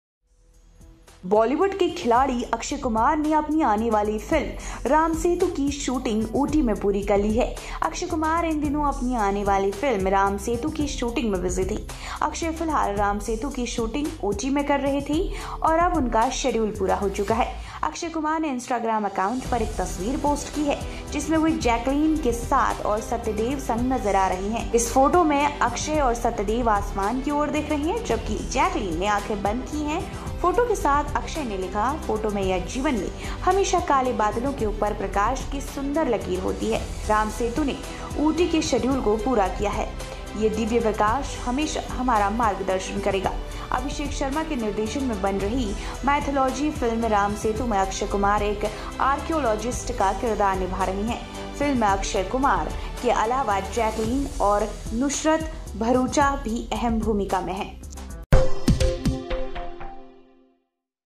AKSHAY KUMAR | BOLLYWOOD NEWS | आने वाली फिल्म रामसेतु शूटिंग ऊटी में पूरी कर ली है
बॉलीवुड के खिलाड़ी कुमार, अक्षय कुमार ने अपनी आने वाली फिल्म रामसेतु शूटिंग  ऊटी में पूरी कर ली है अक्षय कुमार इन दिनों अपनी आने वाली फिल्म रामसेतु की शूटिंग में बिजी थे अक्षय फिलहाल रामसेतु की शूटिंग ऊटी में कर रहे थे और अब उनका शेड्यूल पूरा हो गया है अक्षय कुमार ने इंस्टाग्राम अकाउंट पर एक तस्वीर पोस्ट की है जिसमें वहां जैकलिन फर्नांडीस और सत्यदेव के साथ नजर आ रहे हैं इस फोटो में अक्षय और सत्यदेव आसमान की ओर देख रहे हैं जबकि जैकलिन ने आंखें बंद की हुई है फोटो के साथ अक्षय ने लिखा कि फोटो में या जीवन में हमेशा काले बादलों के ऊपर प्रकाश की सुंदर लकीर होती है रामसेतु के उठी शेड्यूल को पूरा किया आशा है कि दिव्य प्रकाश हमेशा हमारा मार्गदर्शन करेगा l अभिषेक शर्मा के निर्देशन में बनी रही माइथोलॉजी फिल्म रामसेतु में अक्षय कुमार एक आर्कियोलॉजिस्ट का किरदार निभा रहे हैं फिल्म में अक्षय कुमार के अलावा जैकलिन फर्नांडीस और नुशरत भरुचा भी अहम भूमिका में है